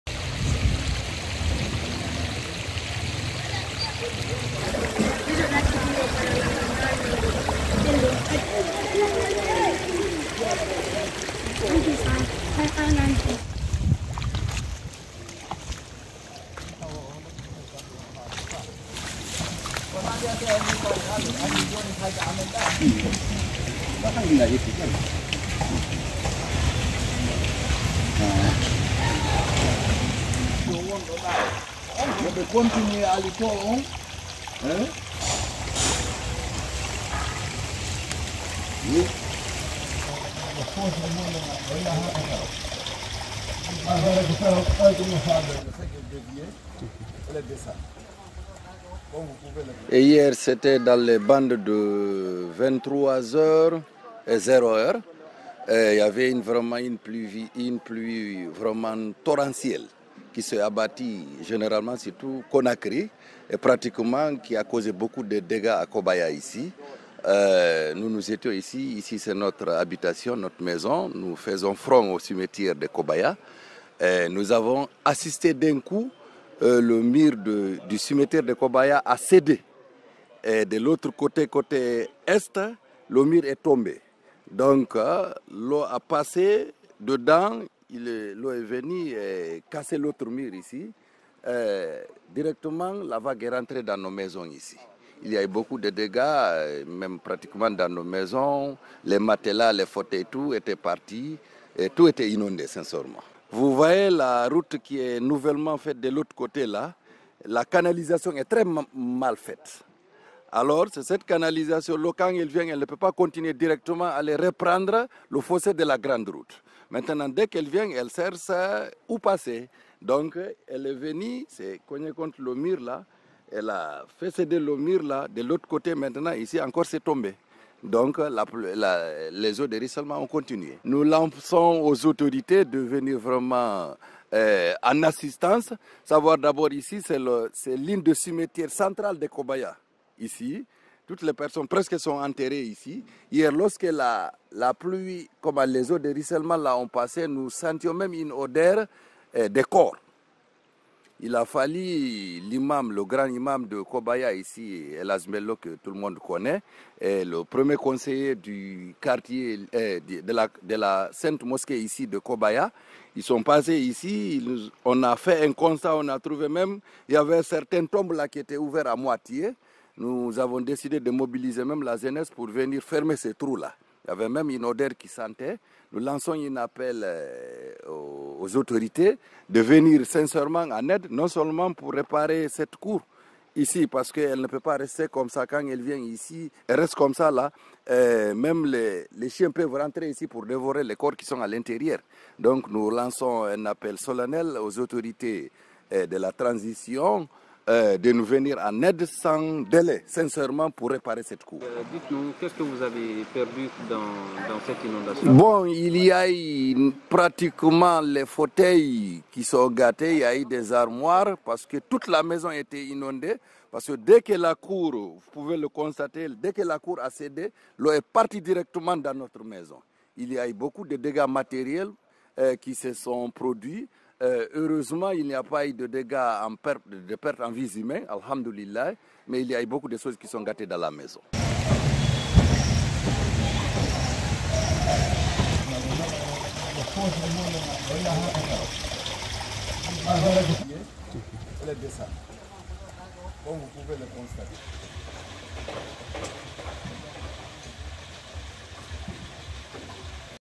SOS pour le Cimetière de Kobaya : certaines tombes sont à moitié ouvertes par les inondations...
La pluie diluvienne qui s'abat sur Conakry depuis hier, lundi 14 août 2023, continue de faire des dégâts. Situé dans la comme de Ratoma, le cimetière de Kobaya n’a pas été épargné par les conséquences de cette pluie torrentielle. Construite pourtant en ciment, la cour de ce cimetière n'a pu résister à la puissante vague d'eau de ruissellement. C’est aux environs de 3 heures du matin qu’elle a finalement cédé aux assauts incessants.